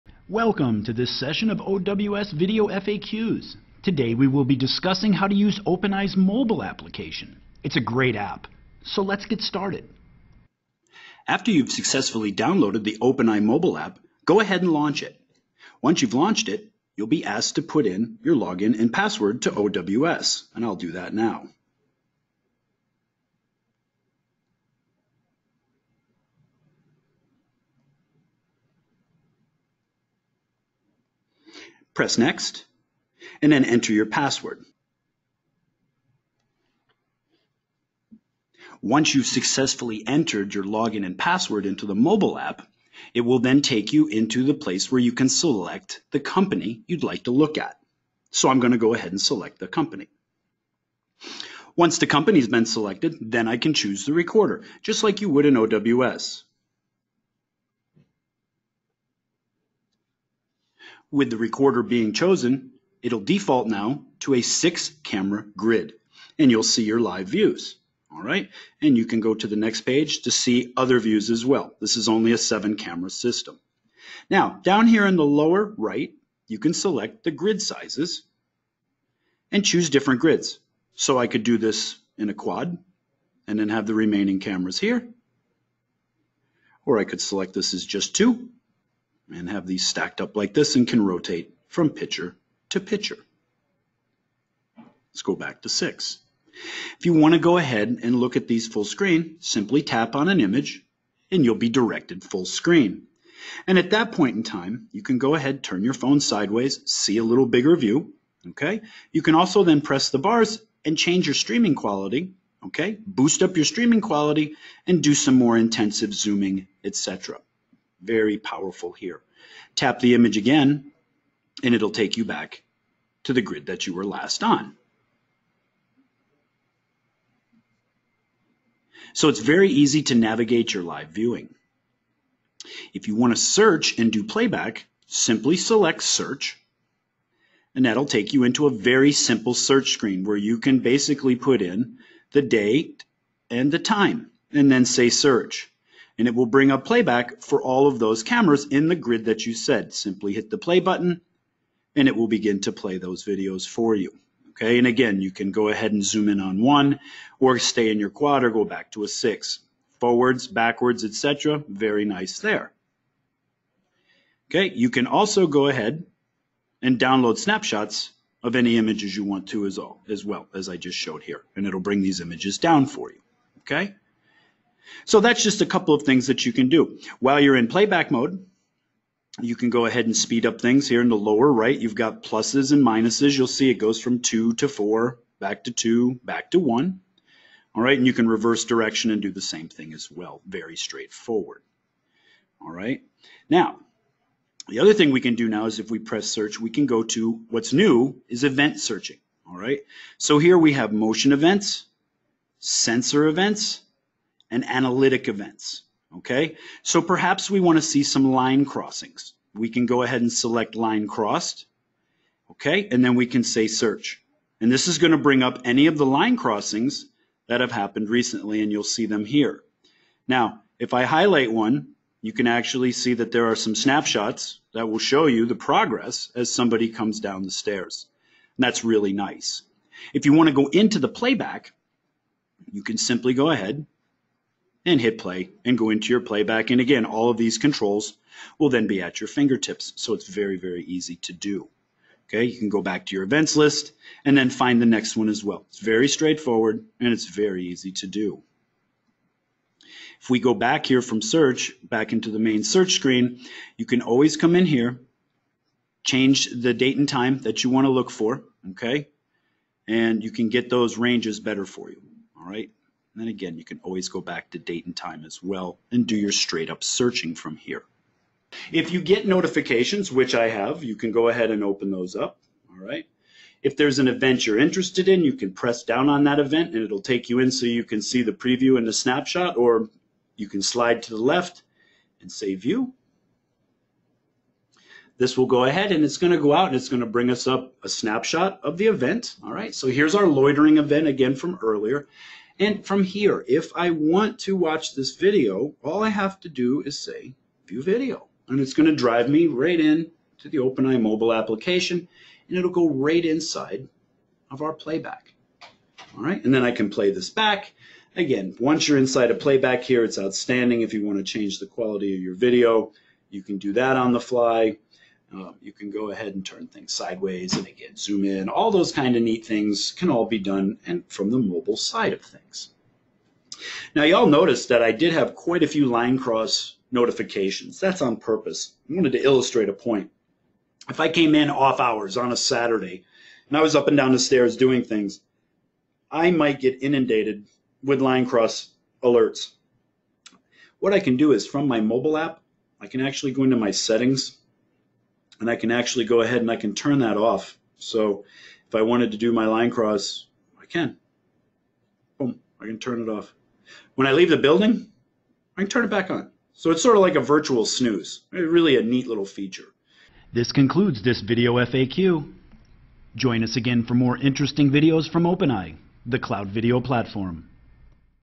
OpenEye Video FAQ - Using OpenEye Mobile App
Learn how to use all the features within OpenEye Mobile